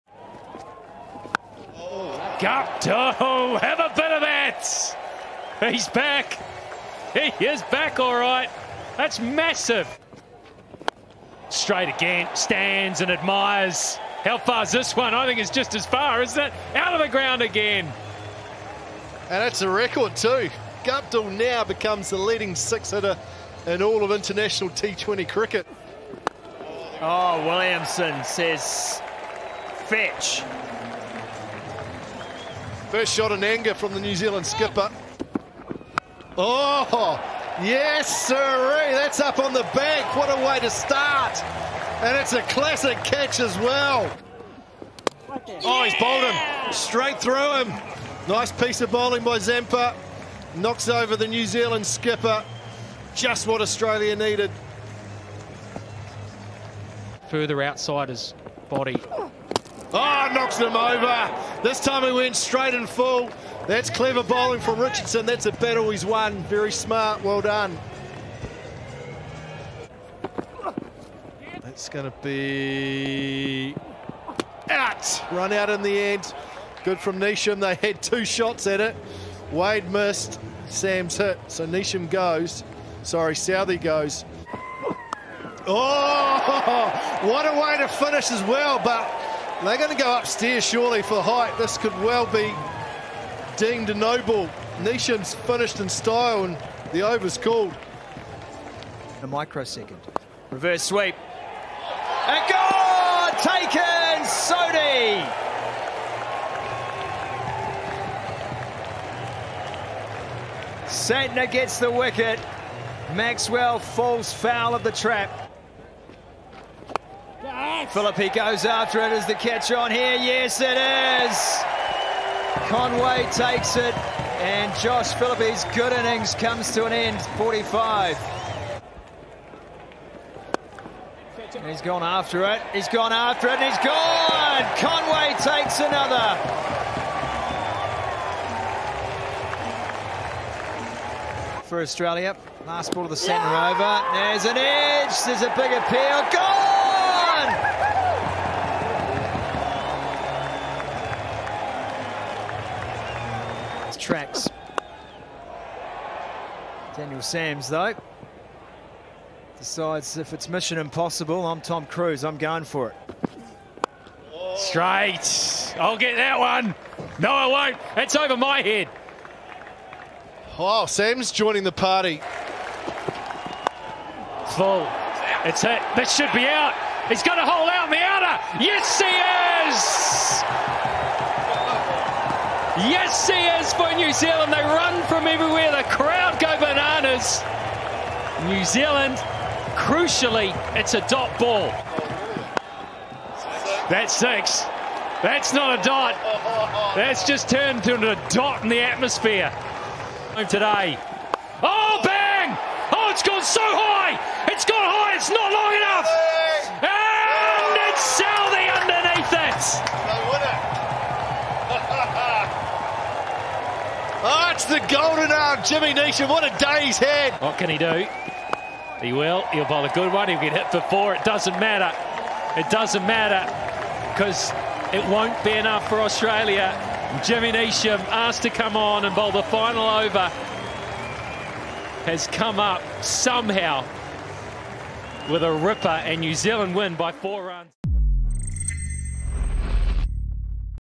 Guptill 97 headlines final over THRILLER | 2nd KFC T20 SHORT HIGHLIGHTS | BLACKCAPS v Australia
Short highlights from the second match of the KFC T20 Series between the BLACKCAPS and Australia at Dunedin's University of Otago Oval.